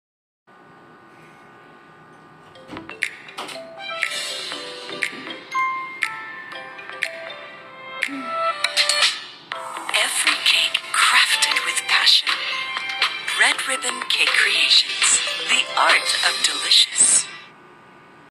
Red Ribbon Cake Creations "Porsche" 15s TVC 2017 (Subtitled)
From the classic favorite Black Forest to the newest indulgence of Cappuccino Creme and Peach Mango Symphony, Red Ribbon has cakes crafted to captivate your senses.

Red Ribbon Cake Creations - - savor the art of delicious.